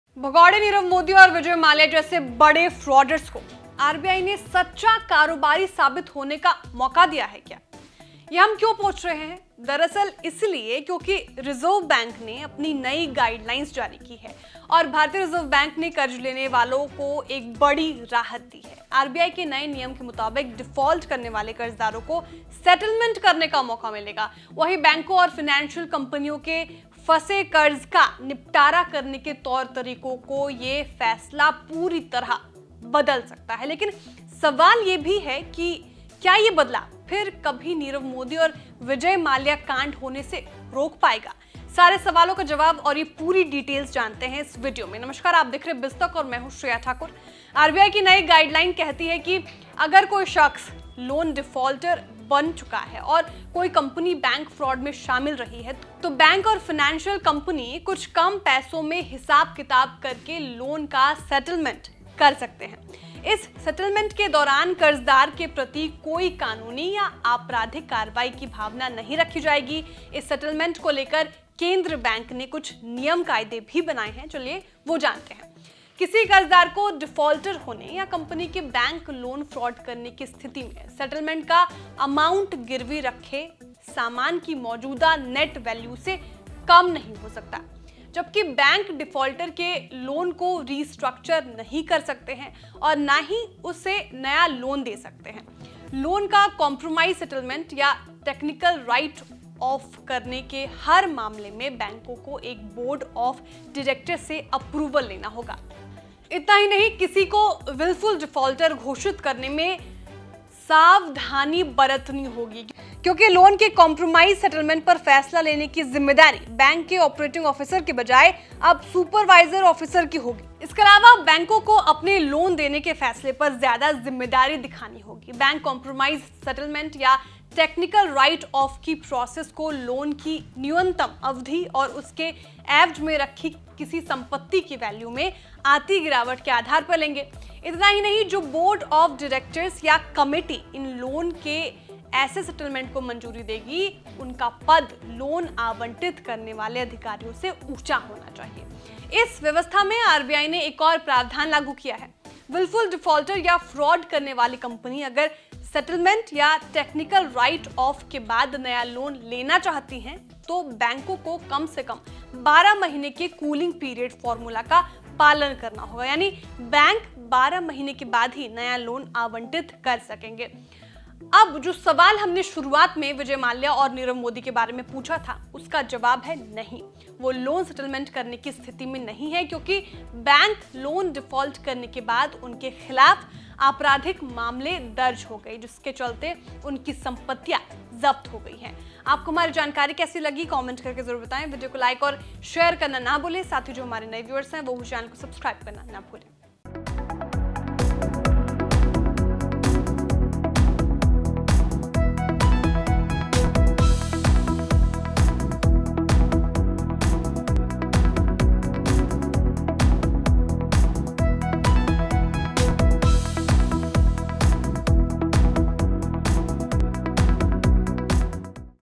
RBI ने लोन लेने वालों के लिए जारी की नई गाइडलाइंस, क्या बड़े भगौड़े अब बच जाएंगे? |BIZ Tak
BizTak Premium ज्वाइन करने के लिए क्लिक करें .